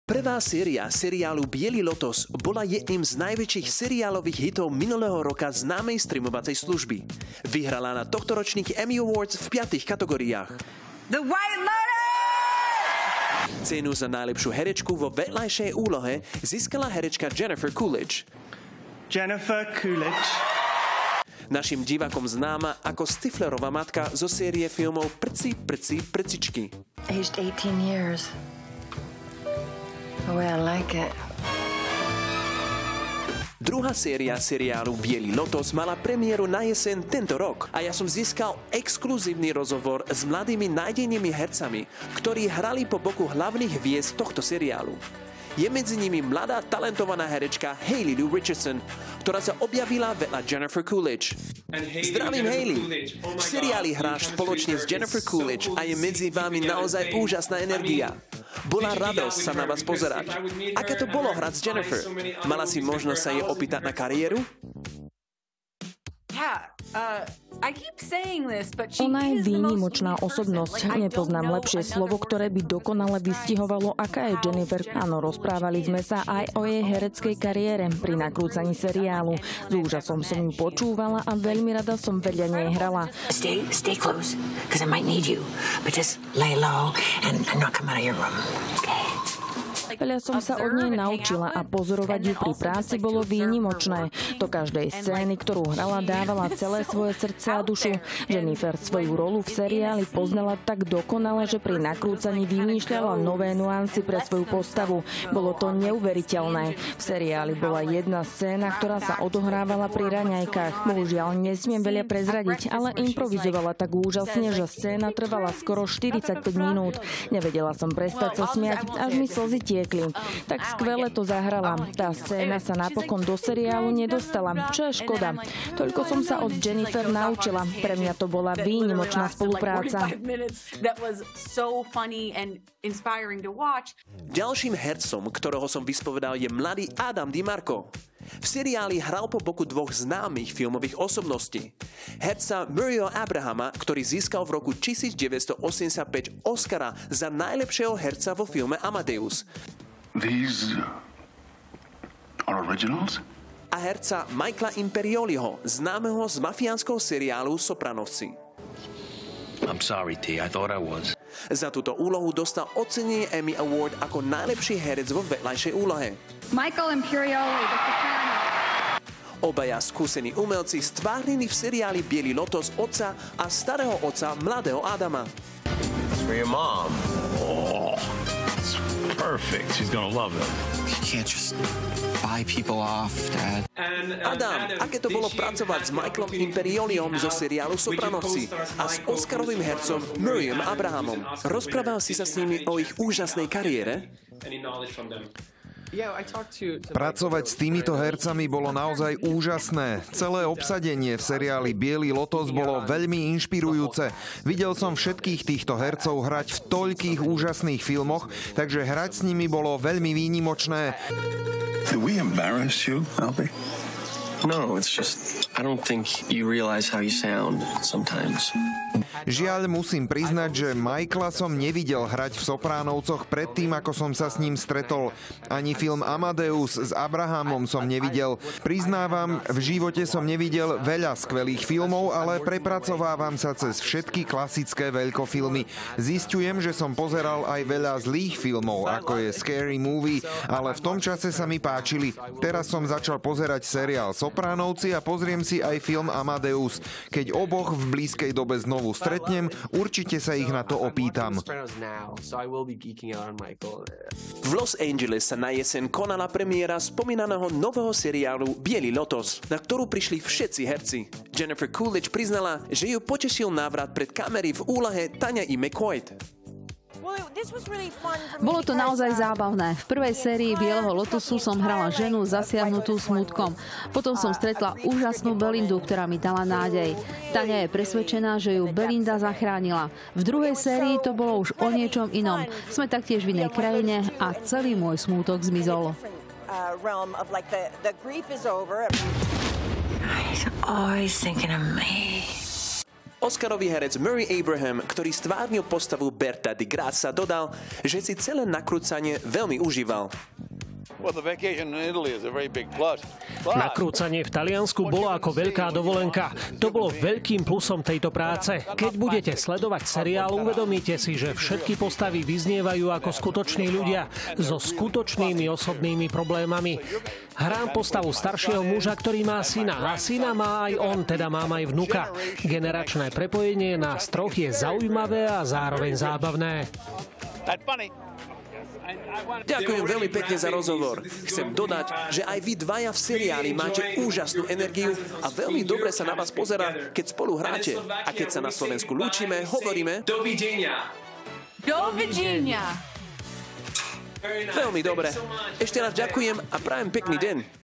Haley Lu Richardson & Adam Di Marco rozhovory | The White Lotus | TV JOJ
Prvá seria seriálu “Biely Lotos” @thewhitelotus bola jedným z najväčších seriálových hitov minulého roka známej streamovacej služby. Vyhrala na tohtoročných Emmy Awards v 5 piatich kategóriách. Cenu za najlepšiu herečku vo vedľajšej úlohe, získala herečka Jennifer Coolidge našim divakom známa ako Stifflerova matka so serie filmov Prci, prci, prcičky. Druhá séria seriálu Biely Lotos mala premieru tento rok a ja som získal exkluzívny rozhovor s mladými nádejnými hercami, ktorí hrali po boku hlavných hviezd tohto seriálu. Je medzi nimi mladá talentovaná herečka Haley Lu Richardson @haleyluhoo a ďalším hercom je mladý Adam Di Marco @adamdimarco.

copyright to TV JOJ (Slovak Republic)